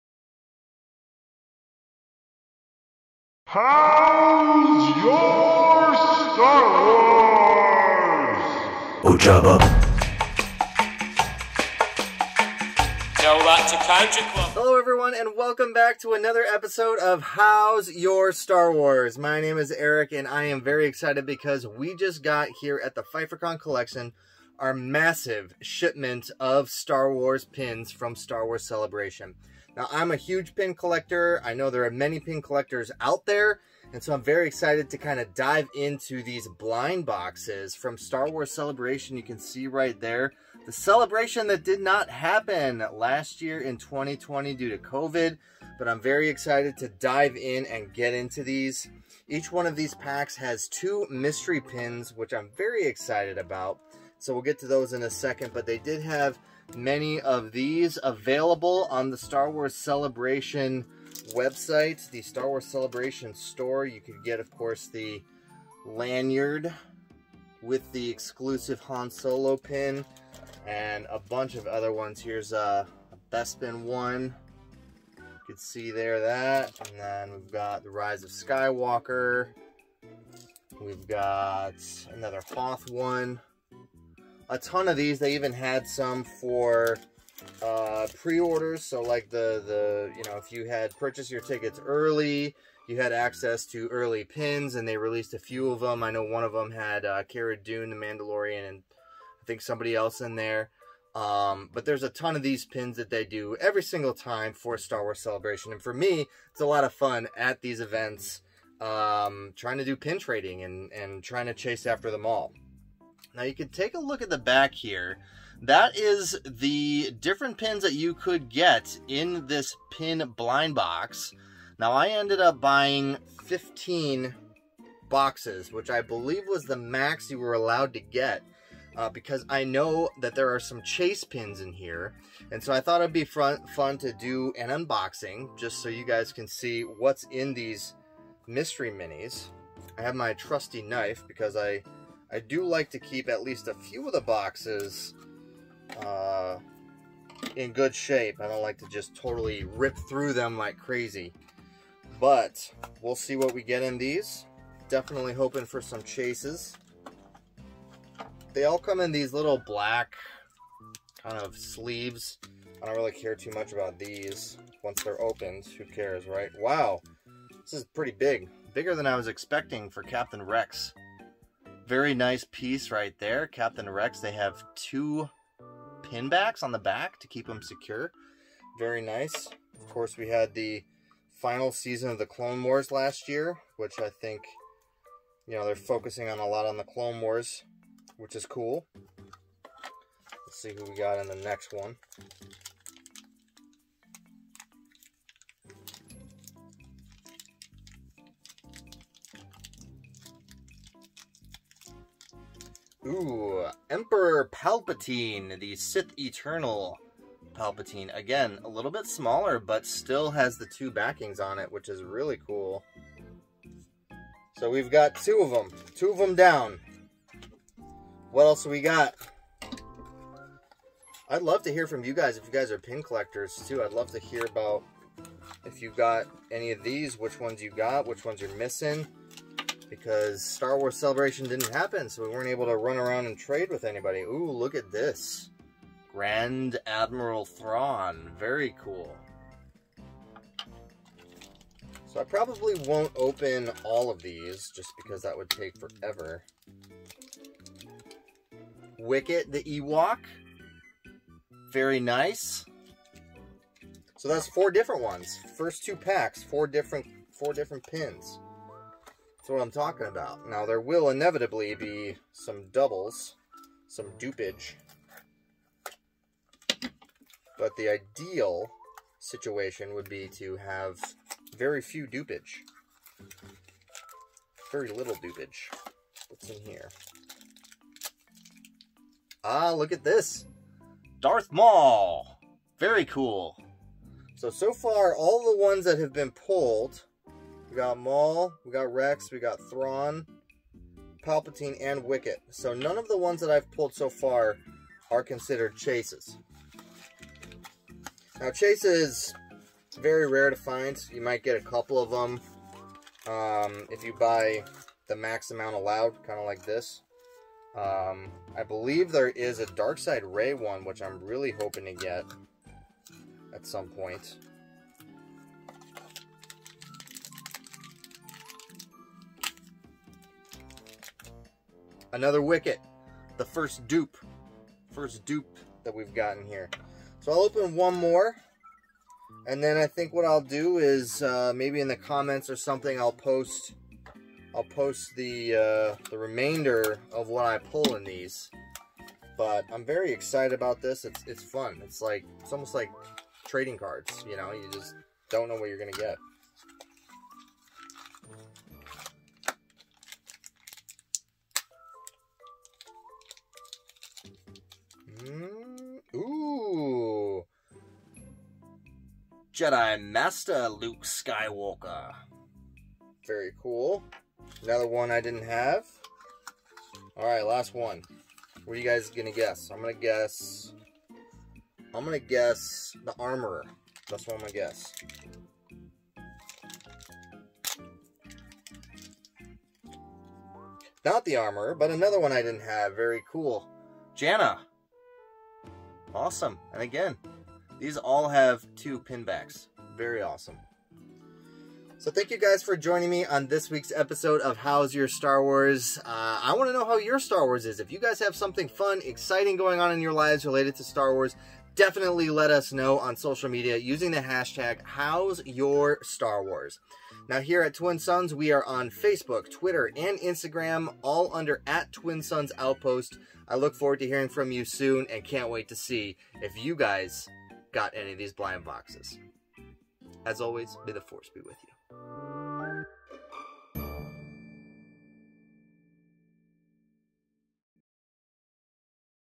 How's Your Star Wars Episode 36 - Eric
On this week's episode of How's Your Star Wars, Eric unboxes some of the pin blind boxes from last year's Star Wars Celebration.  Which pins did you get?